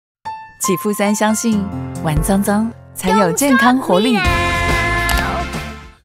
【啟賦3 玩髒髒讓寶寶更健康有活力！】6s
你是否常說「髒髒，不可以」
其實玩髒髒讓寶寶更健康有活力！

讓孩子髒玩，是尊重孩子的選擇，是他們接觸探索最真實世界的好機會！
啟賦3多元活性因子，能開啟寶寶健康活力
讓媽媽更敢放手，和寶寶一起玩髒髒